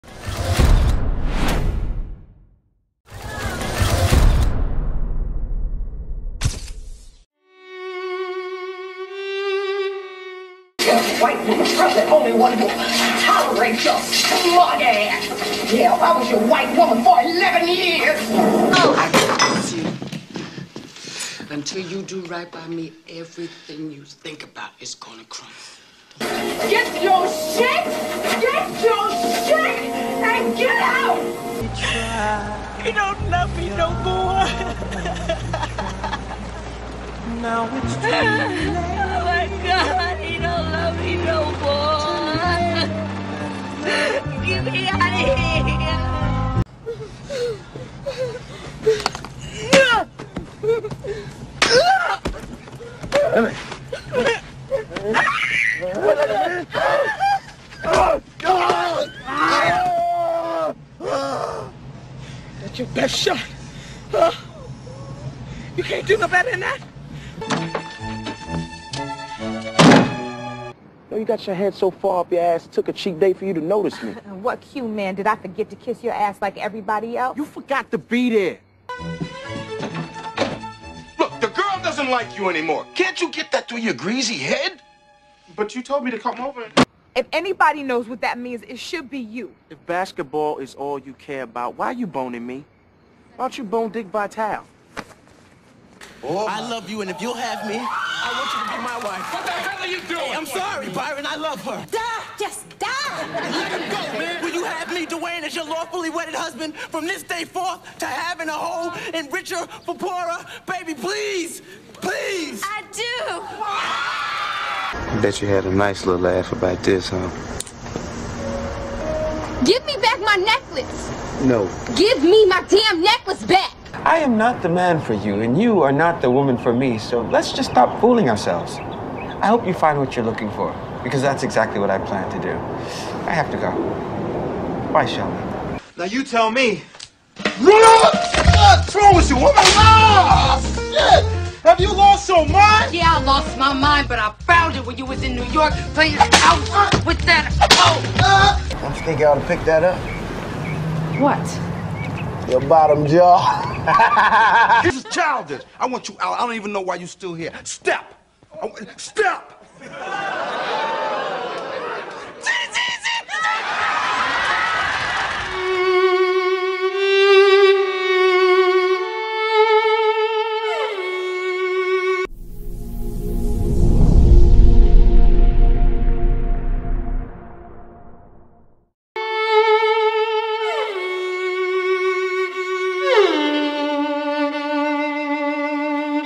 Black Hollywood's Greatest Breakups | BOSSIP
The greatest breakups of Black Hollywood.

In honor of Valentine's Day we put together a mashup of Black Hollywood's greatest breakup scenes. Of course there's more but we couldn't put them all in one video!